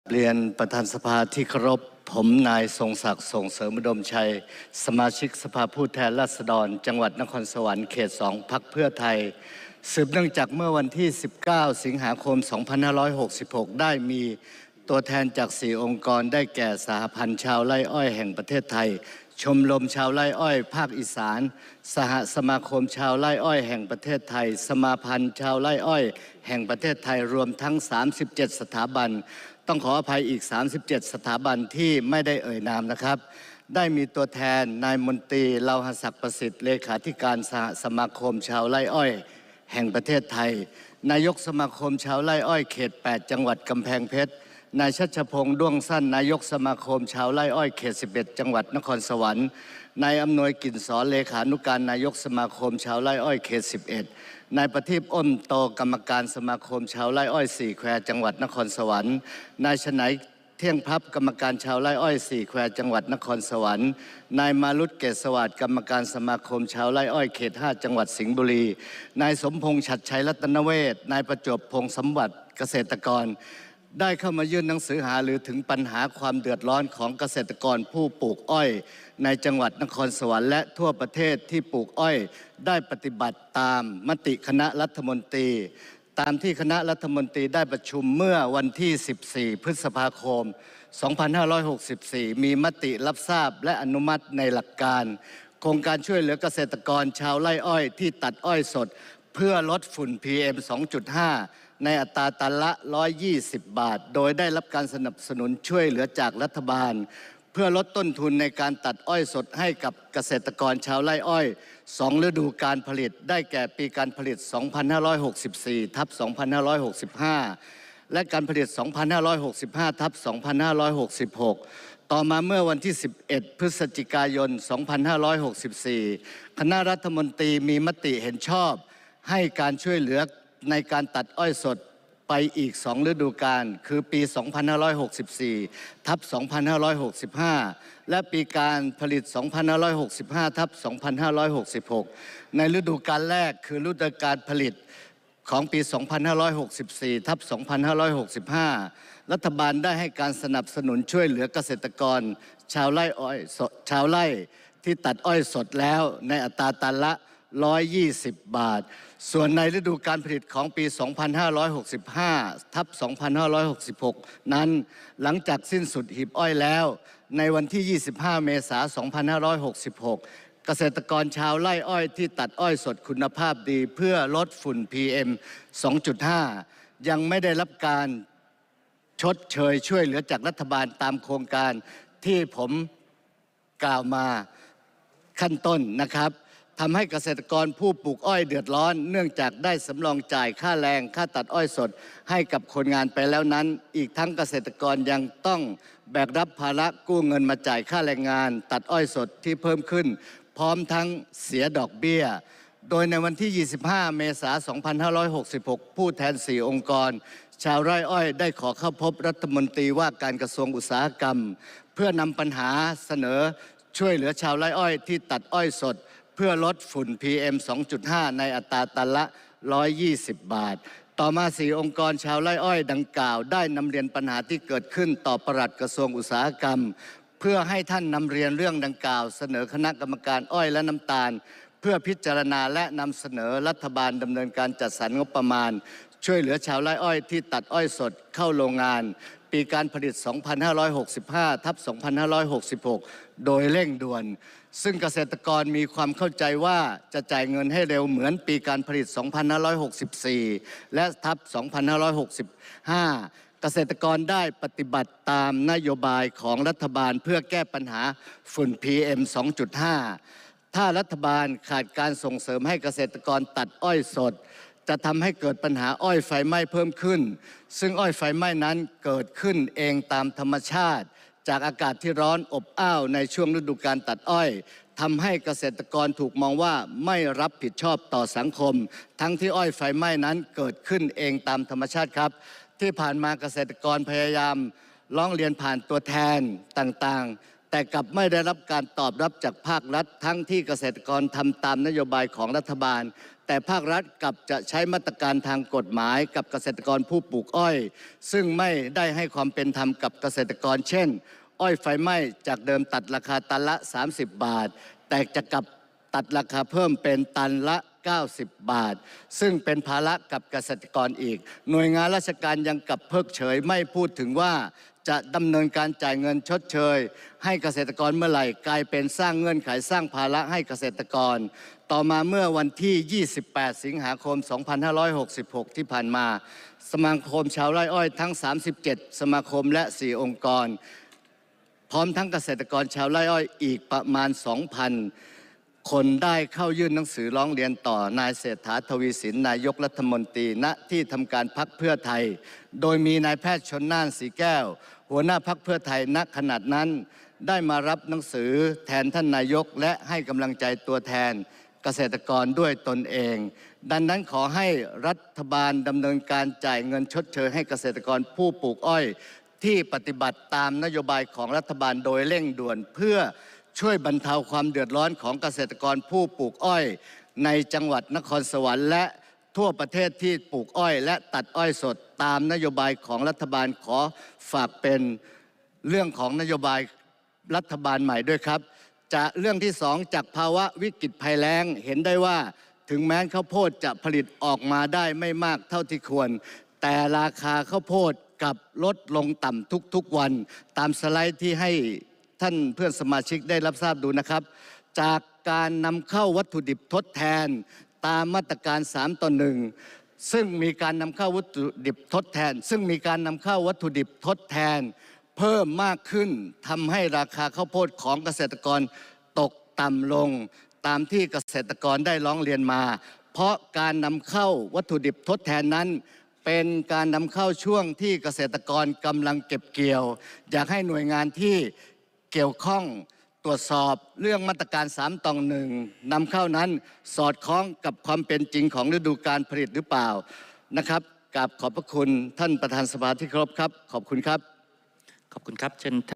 ทรงศักดิ์ ส่งเสริมอุดมชัย
การประชุมสภาผู้แทนราษฎรชุดที่ 26 ปีที่ 1 ครั้งที่ 13 (สมัยสามัญประจำปีครั้งที่หนึ่ง)
วันพุธที่ 6 กันยายน พ.ศ. 2566

     - ญัตติ เรื่อง ขอให้สภาผู้แทนราษฎรตั้งคณะกรรมาธิการวิสามัญพิจารณาศึกษาแนวทางการแก้ไขปัญหาราคากุ้งตกต่ำ
     - ญัตติ เรื่อง ขอให้สภาผู้แทนราษฎรพิจารณาการแก้ไขปัญหาราคาพืชผลทางการเกษตรตกต่ำและต้นทุนสูง
     - ญัตติ เรื่อง ขอให้สภาผู้แทนราษฎรพิจารณาศึกษาแก้ไขปัญหาราคาผลผลิตตกต่ำอย่างเป็นระบบ
     - ญัตติ เรื่อง ขอให้สภาผู้แทนราษฎรตั้งคณะกรรมาธิการวิสามัญพิจารณาศึกษาแก้ไขปัญหายางพาราตกต่ำอย่างครบวงจรและยั่งยืน ฯลฯ